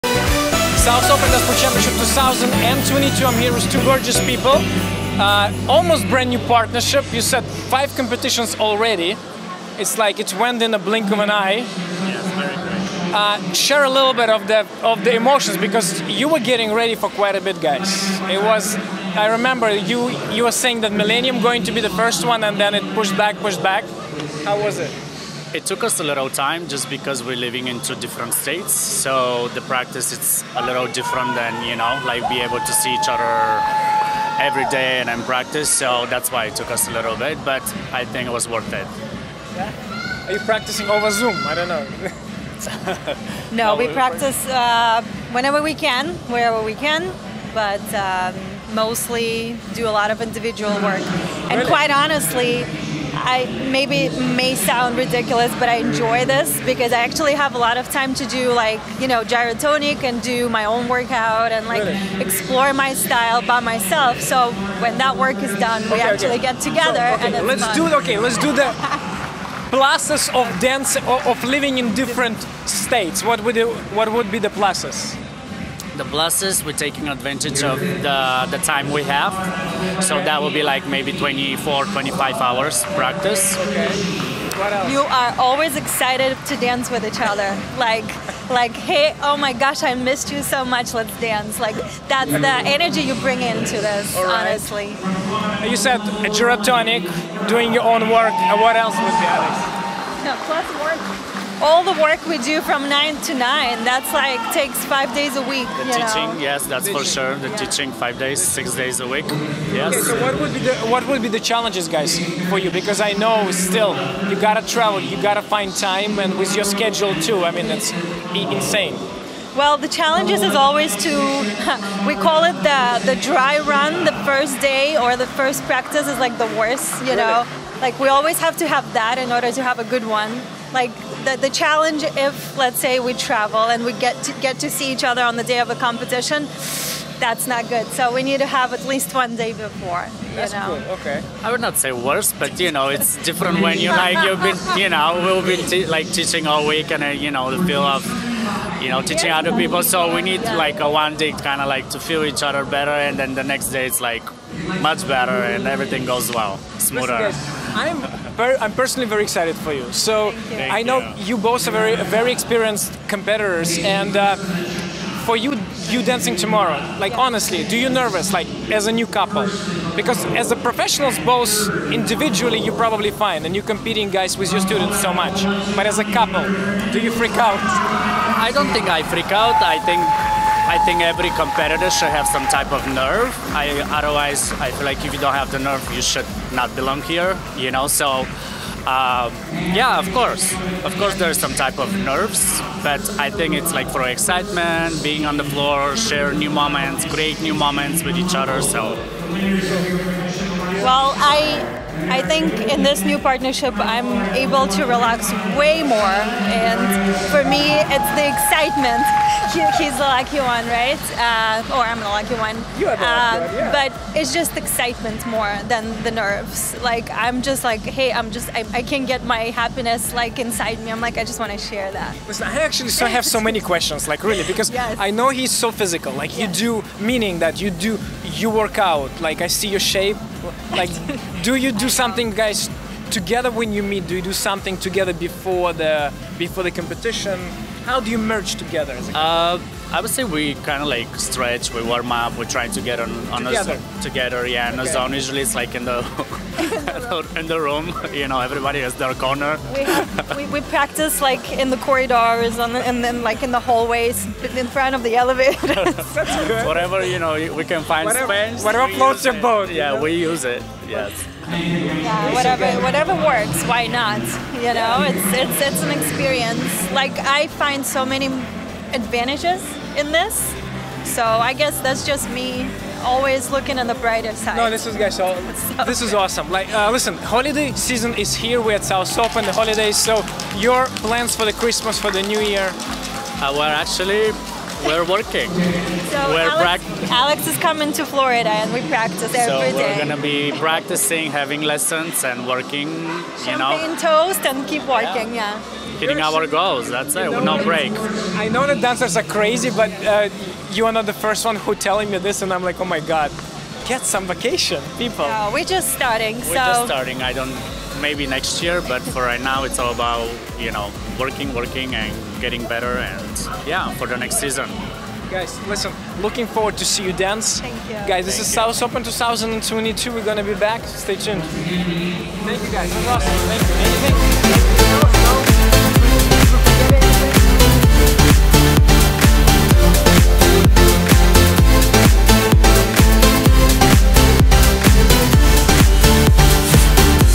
Aleks Nashev and Evgenia Elcoro about long distance relationships
South Open Dancesport Championships 2022. Aleks Nashev and Evgenia Elcoro about long distance relationships in dance partnerships
South Open Dancesport Championships: